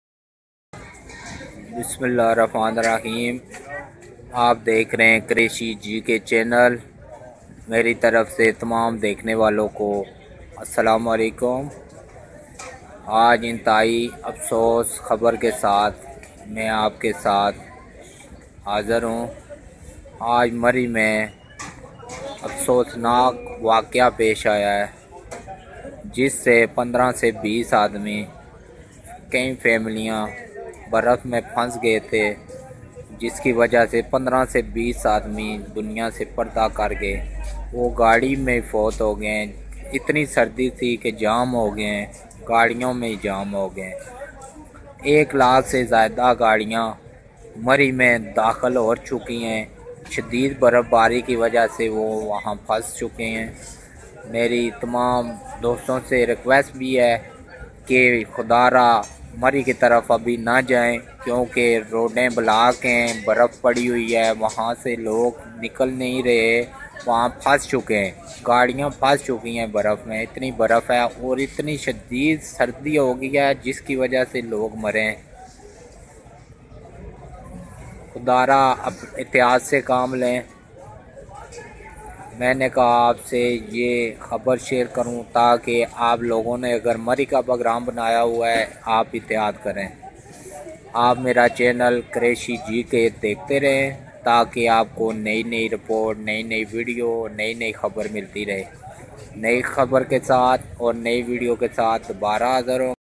مری افسوس ناک واقعہ لوگ برف میں پھنس گےجس میں بیس آدمی بازی ہار بیٹھے گاڑیوں میں جام ہوں گے
mary new update # dead body Mary#mary 2022#